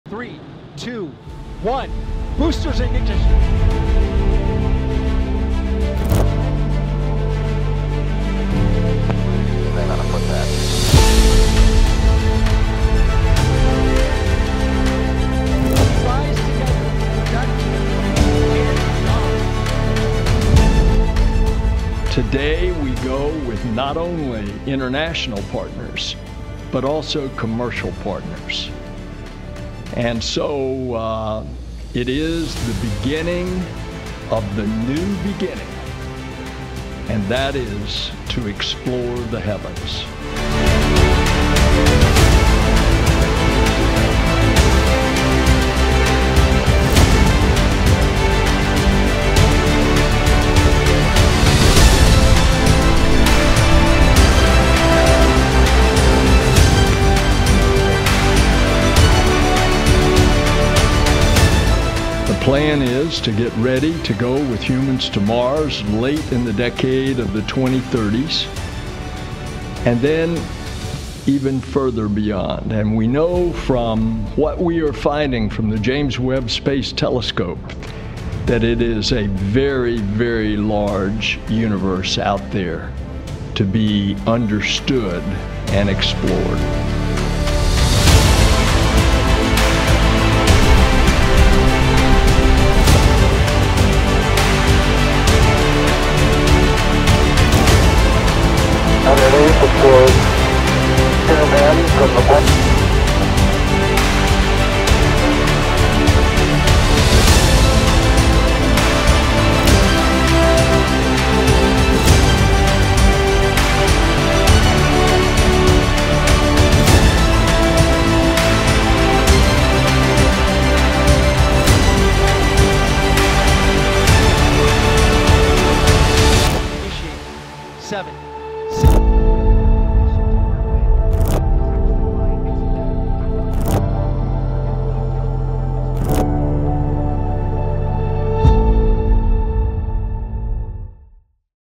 نجاح مهمة "أرتميس 1" إلى القمر.. و"ناسا": "بداية جديدة لاستكشاف السماء"
"ناسا" تعلن نجاح مهمة "أرتميس 1" إلى القمر في خطوة سيتبعها إرسال 4 رواد فضاء عام 2024 إلى القمر يليها شخصين إلى المريخ